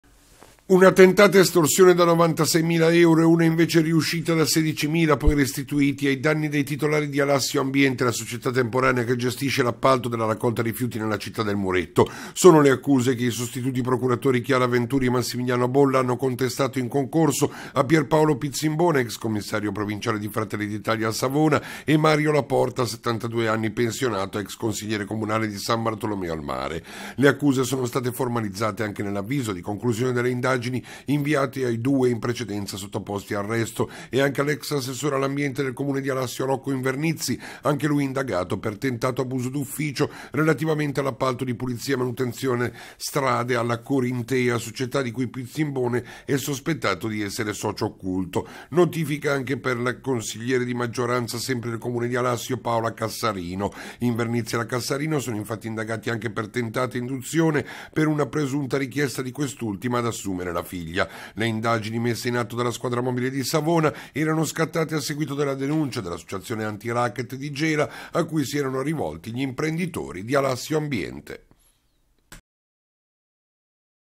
CONCLUSE AD ALASSIO LE INDAGINI SU PIZZIMBONE, LA PORTA, INVERNIZZI E CASSARINO
Una tentata estorsione  da 96 mila euro e una invece riuscita  da 16 mila euro, poi restituiti, ai danni dei titolari di Alassio Ambiente, la società temporanea che gestisce l’appalto della raccolta rifiuti nella città del muretto. Sono le accuse che i sostituti procuratore Chiara Venturi e Massimiliano Bolla hanno contestato in concorso a Pier Paolo Pizzimbone, ex commissario provinciale di Fratelli d’Italia e Mario La Porta, 72 anni, pensionato ed ex consigliere comunale di San Bartolomeo al Mare. Le accuse sono state formalizzate anche nell’avviso di conclusione delle indagini inviato ai due in precedenza sottoposti ad arresto e anche all’ex assessore all’ambiente del Comune di Alassio Rocco Invernizzi, anche lui indagato per tentato abuso d’ufficio, relativamente all’appalto di pulizia e manutenzione strade alla Co.R.in.t.e.a., società di cui Pizzimbone è sospettato di essere socio occulto. Notifica anche per consigliere di maggioranza sempre del comune di Alassio, Paola Cassarino. Invernizzi e la Cassarino sono indagati per tentata induzione per una presunta richiesta di quest’ultima ad assumere la figlia. Le indagini messe in atto dalla Squadra mobile di Savona erano scattate a seguito della denuncia dell’associazione Antiracket di Gela a cui si erano rivolti gli imprenditori di Alassio Ambiente.